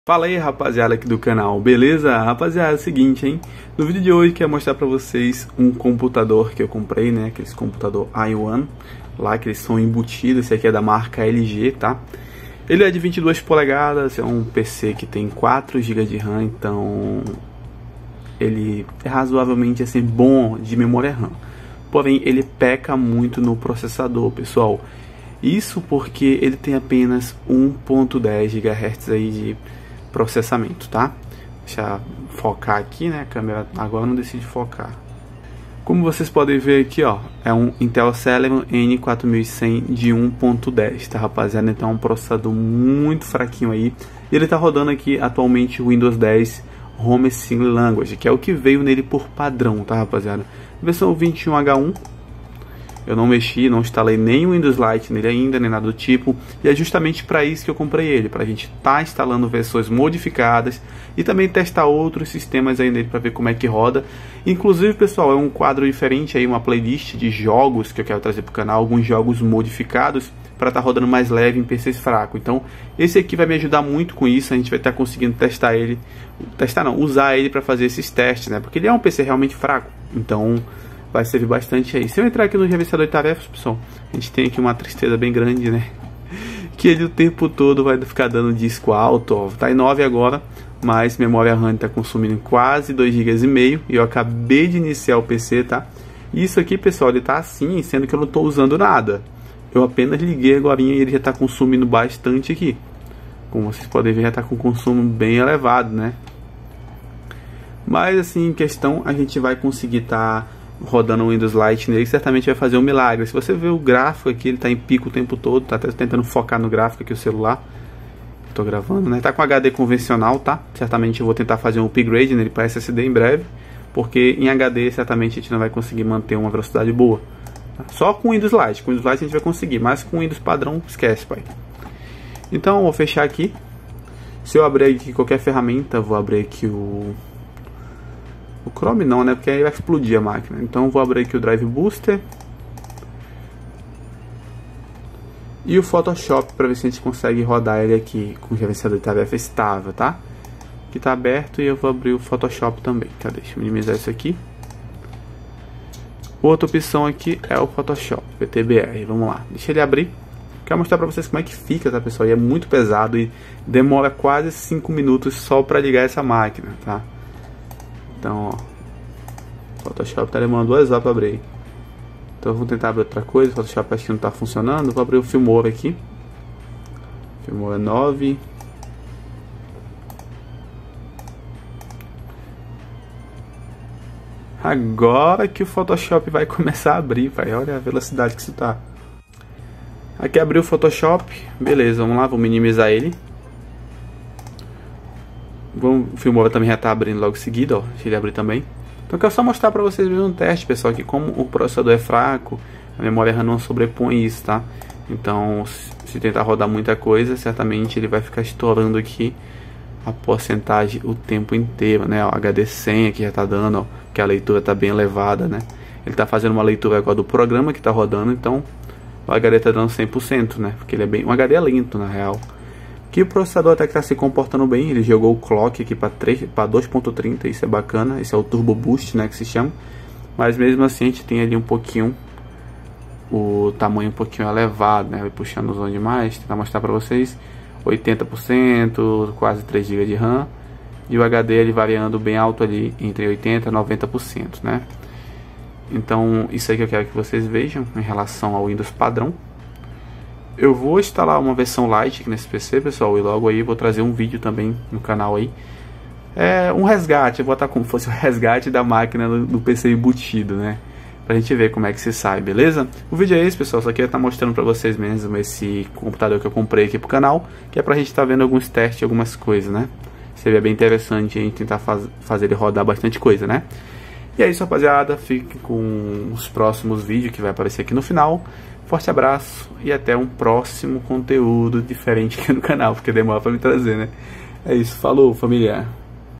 Comprei um PC All IN One LG 22V280 Para Testes
Comprei um PC All IN One LG 22V280 Para Testes de sistemas e alguns jogos para pc fraco aqui no canal, o computador All IN One LG 22V280 é um pc que passou muito tempo no mercado e sinceramente tem um desempenho de pc ddr2 mesmo sendo um ddr4, então peguei ele para estar colocando alguns sistemas operacionais modificados para ver como ele se sai

● VÍDEOS SEMPRE

●COMPRE OS MESMOS ACESSORIOS QUE EU USO ●
● BAJEAL K100 Mecânico Teclado

● USE O CUPOM: BG9ee774

 Xerife Tech ©